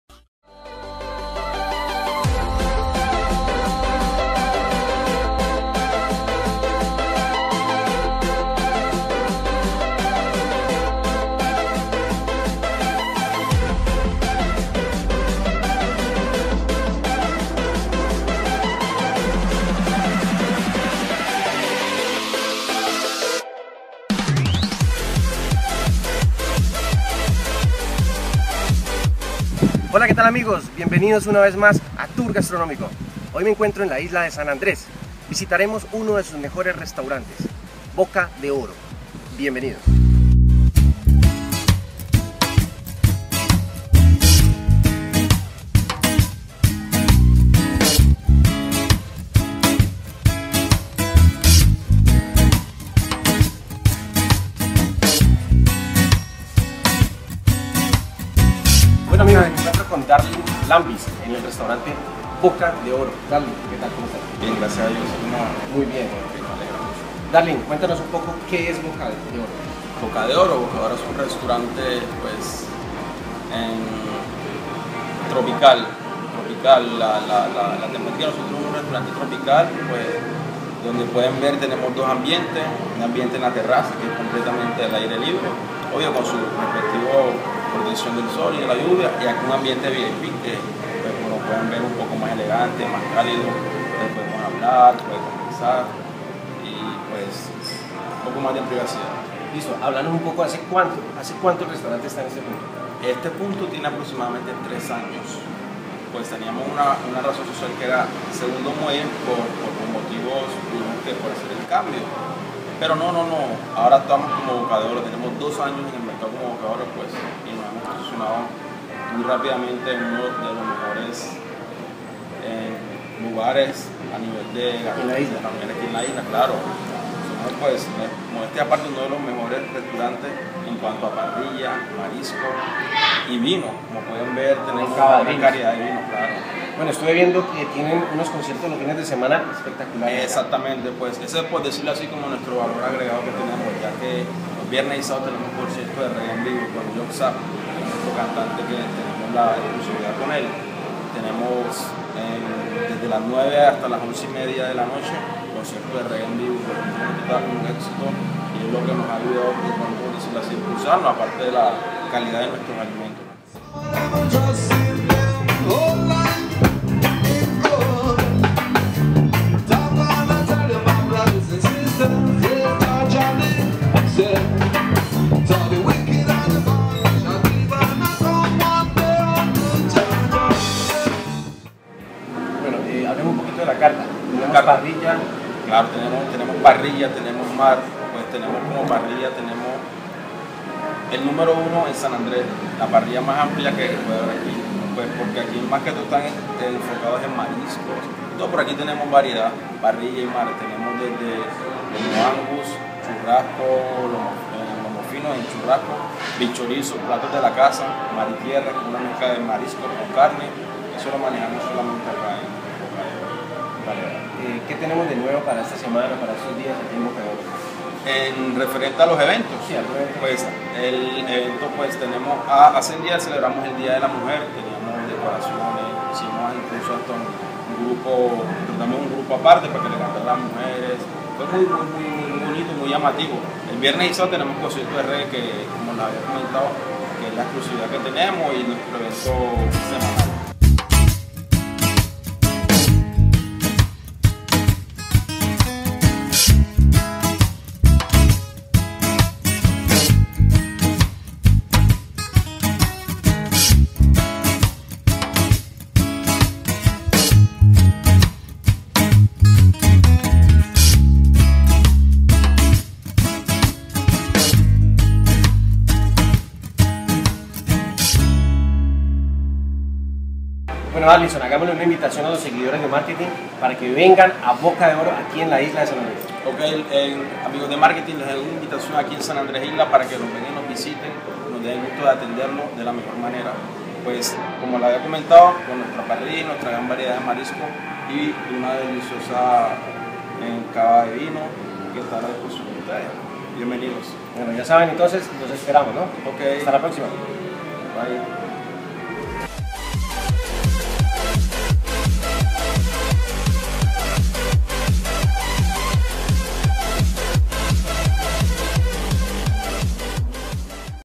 Restaurante Bocca de Oro - Tour Gastronómico – Episodio 7
De tránsito por nuestra hermosa isla de San Andrés no podíamos dejar pasar la oportunidad para traerles un TourGastronomico en éste paradisiaco rincón de la geografía. En ésta oportunidad visitamos a Boca De Oro Bar Restaurante uno de los mejores restaurantes de la isla y poseedores de una de las parrillas más completas del lugar, encontramos mar y tierra conjugados en sus platos y acompañado de una excelente selección de vinos... Y claro no podía faltar el Reggae como complemento de una fantástica experiencia caribeña.
Imagenes proporcionadas por Bocca de Oro (Caribbean Vision)

Marketing Gastronómico presente en la isla de San Andres, en uno de sus mejores restaurantes, Bocca de Oro.

Estrategia Digital
Estrategias Gastronomicas
Régimen Operativo.
Capacitación del personal en servicio al cliente.